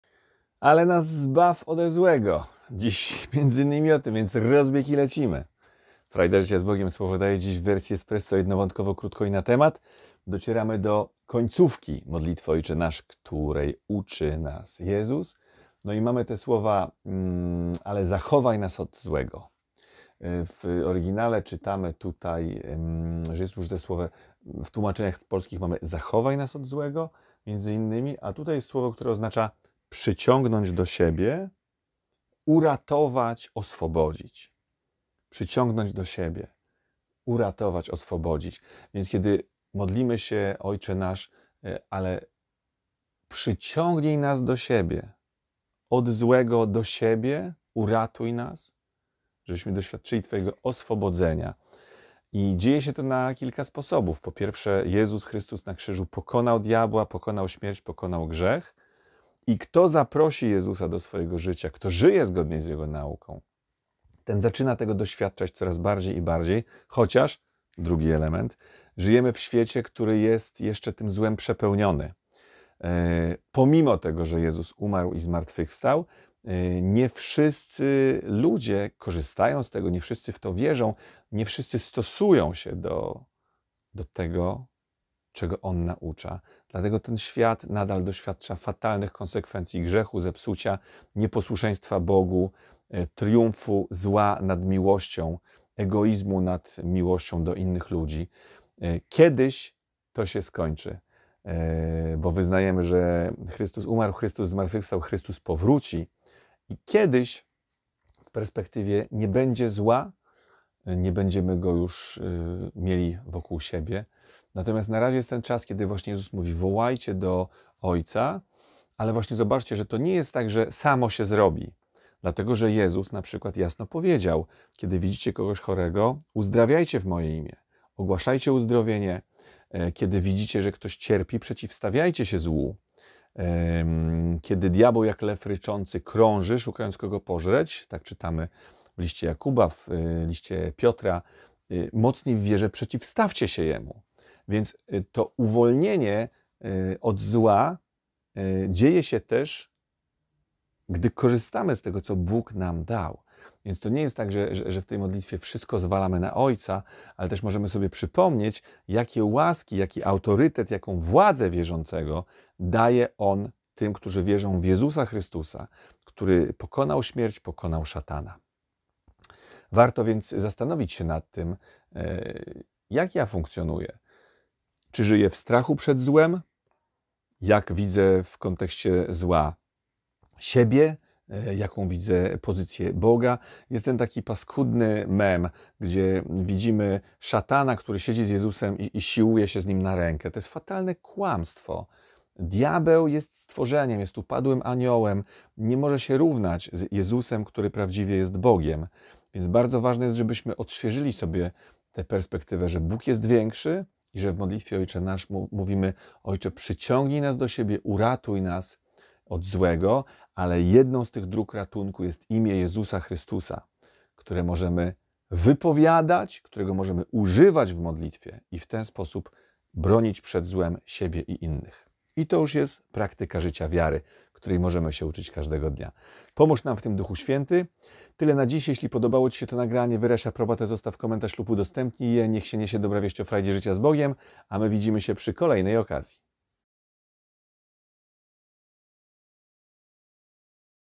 Letnie studium modlitwy "Ojcze Nasz" (cz. 15) - "Frajda życia z Bogiem" (odc. 457)
Zaczynamy letnie studium modlitwy "Ojcze Nasz"!
Drodzy Widzowie! Jeśli podoba Wam się to, co robię, możecie mnie wesprzeć!